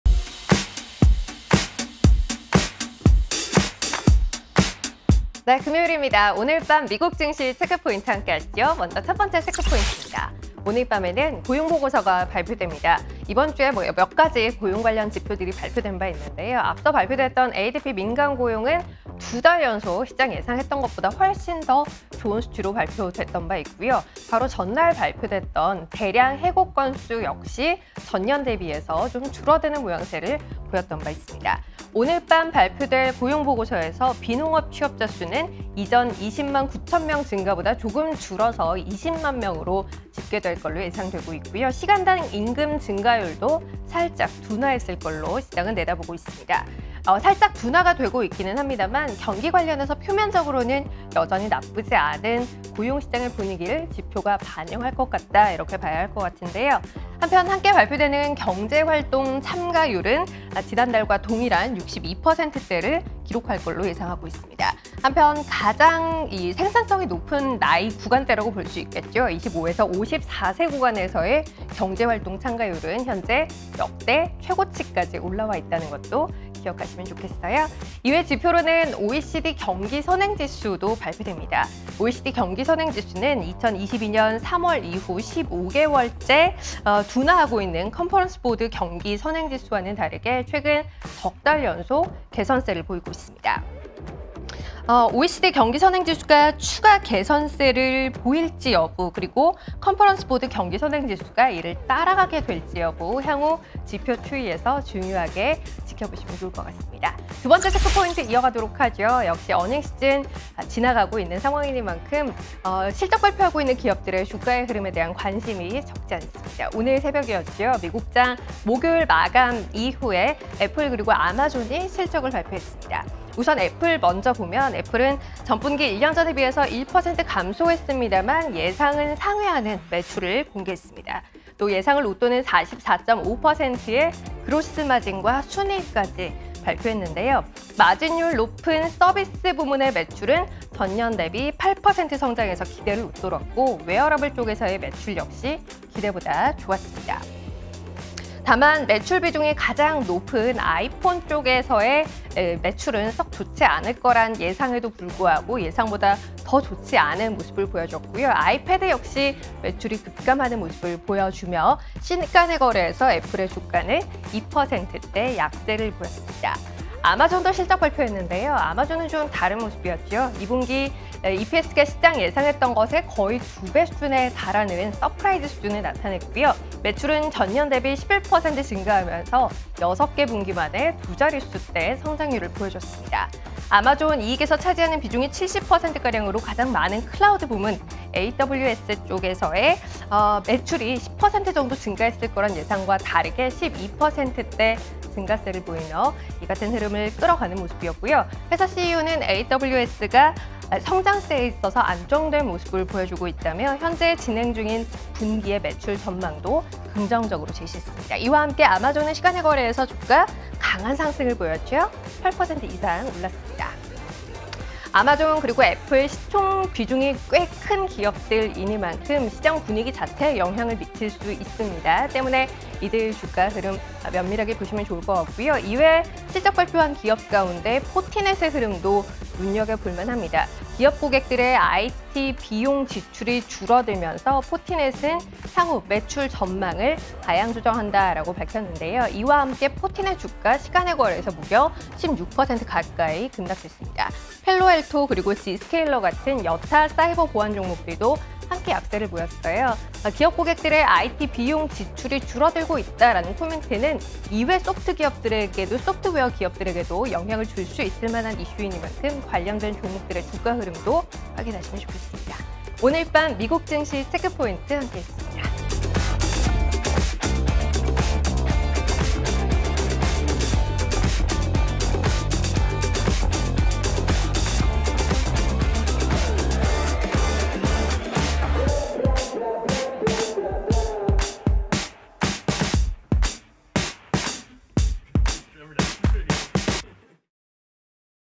[미국증시 체크포인트] 포티넷 시간외 급락, 사이버 보안주 영향은? | 2023.8.4 (금)
오늘의 미국증시 체크포인트

00:08 체크포인트① : 고용보고서
01:46 체크포인트② : 시간외 주가

▼디지털리서치보고서 보기▼
[주식시장 리뷰와 미 증시 전망]

※ 보고서가 열리지 않는 경우, 다른 브라우저로 열어보시기 바랍니다.(인터넷 익스플로어, 크롬)

※ 스마트머니 채널을 구독하시면 실시간 방송 알림을 받아 보실 수 있습니다.